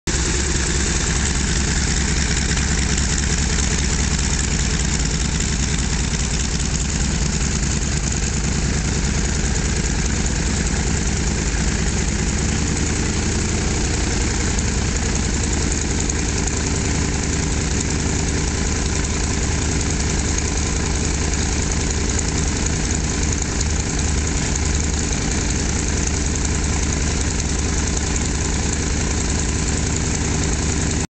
Flugtage Bad Sassendorf / Lohne (2008)
Eine Yak 11 rollt zum Tanken